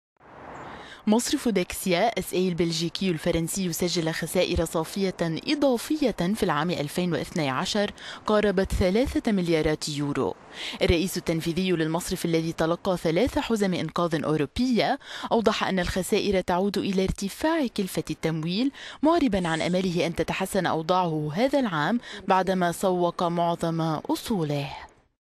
خسائر صافية إضافية لمصرف ديكسيا
مصرف ديكسيا SA البلجيكي الفرنسي سجل خسائر صافية إضافية في العام ألفين وإثني عشر قاربت ثلاثة مليارات يورو.

الرئيس التنفيذي للمصرف الذي تلقي ثلاث حزم إنقاذ أوروبىة أوضح أن الخسائر تعود إلى ارتفاع كلفة التمويل معربا عن أمله أن تتحسن أوضاعه هذا العام بعدما سوق معظم أصوله.